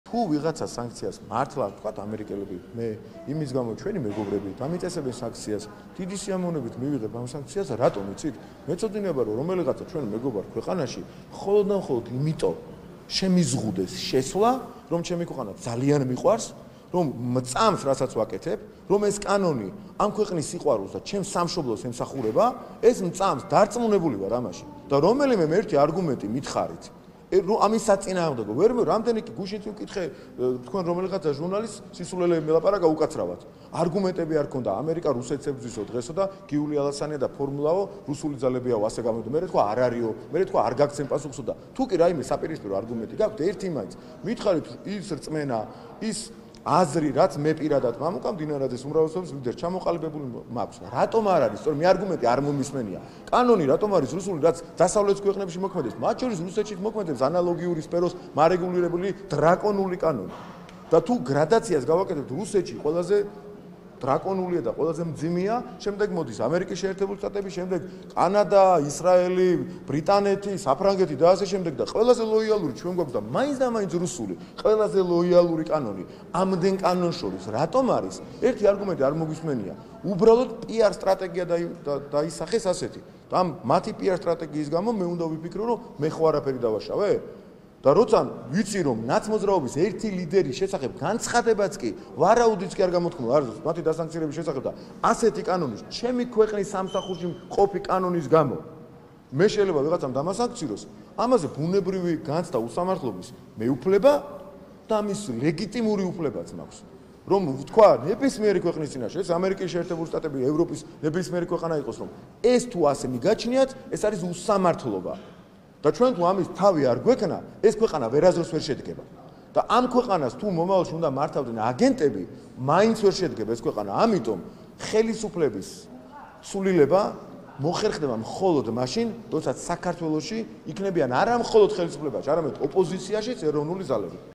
მამუკა მდინარაძე „გამჭვირვალობის კანონზე"
სამშობლოს დაცვის სიყვარულით გამსჭვალული კანონის მიღება ჩვენი მეგობრებისთვის თუ სანქციების საბაბია, ეს ბუნებრივია ქვეყანაში უსამართლობის განცდას აჩენს - მამუკა მდინარაძე